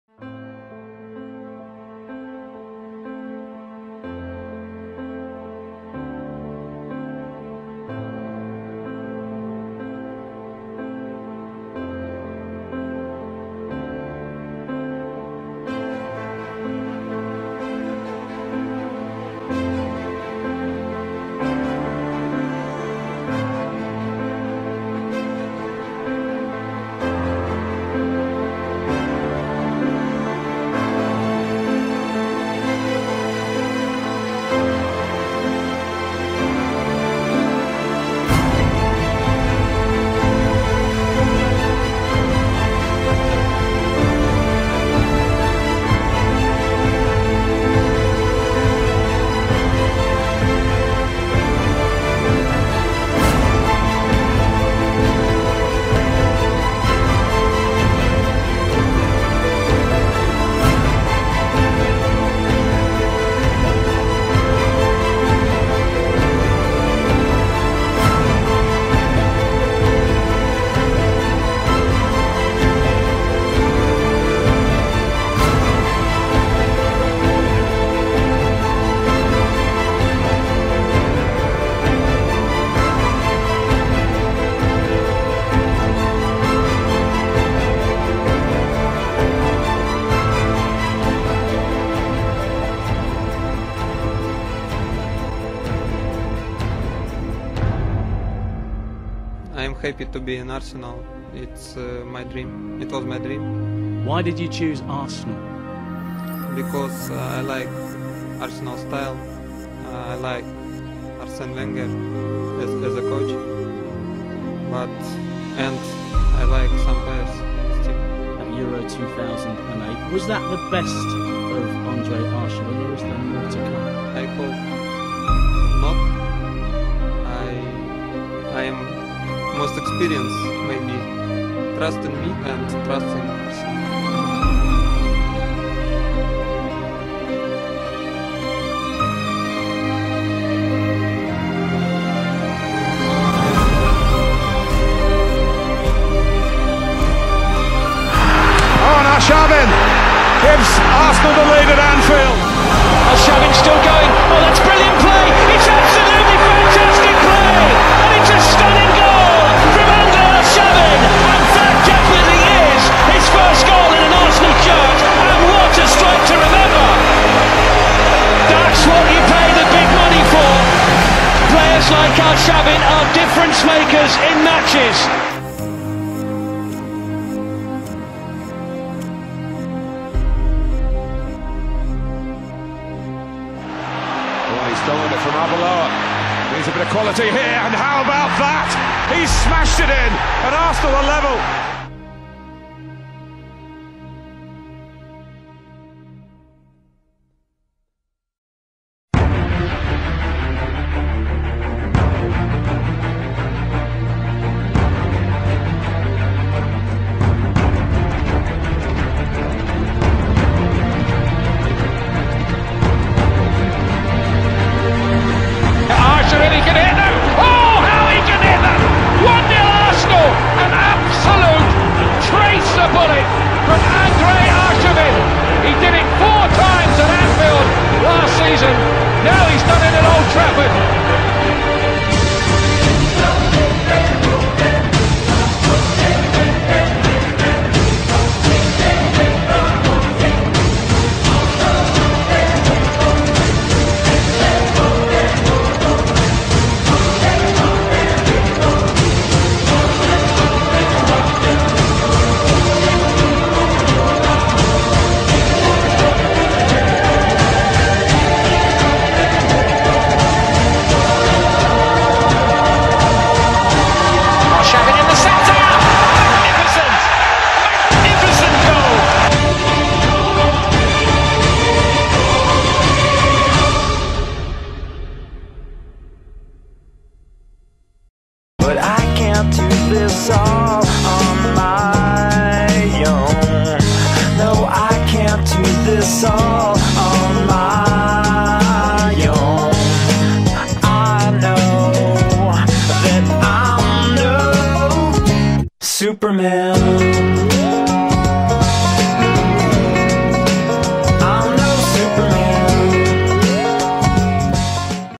The Story of Andrey Arshavin // Goals, Skills, Passes and Close-Ups
Andrey Arshavin, 28, is a Russian winger that currently plays for Arsenal FC, which he joined in the winter of 2009 for a fee believe to be in the region of 15 million Euros. Arshavin was quick to make an impact after joining the English club, and has since cemented himself in the left winger position, becoming a key figure in Arsenal's line-up.

This compilation tells his story, since before he joined Arsenal and up till the current time, showing goals, skills, passes, free-kicks and close-ups of the winger.

I did this in a larger span of time than I usually take to make other compilations, and I believe it turned out pretty well, as it's probably one of my best ones. So, I genuinely hope you like it, and that if you didn't know much about Andrey, you do now. Please don't be reluctant to comment on the compilation, as I read and appreciate every one.

Thanks.